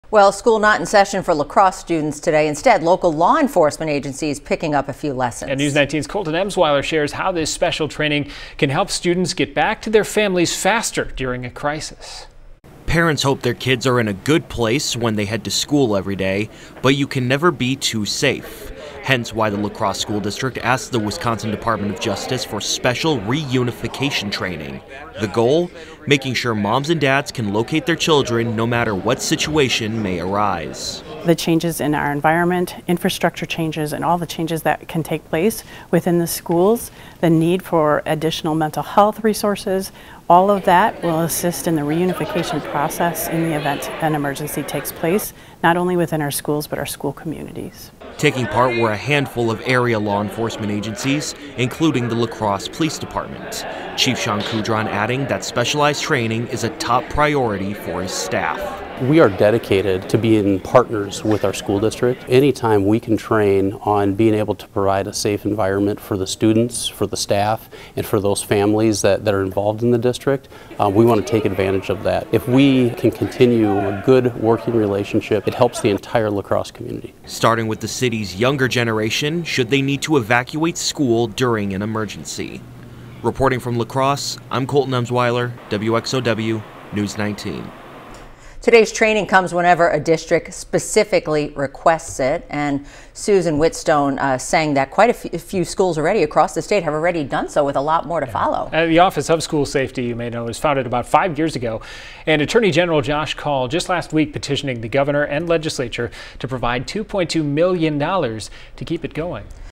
Law enforcement agencies receive DOJ reunification training
School may have been out of session at Logan High School on Monday, but that didn't stop several officers in the area from receiving special training from the Wisconsin Department of Justice.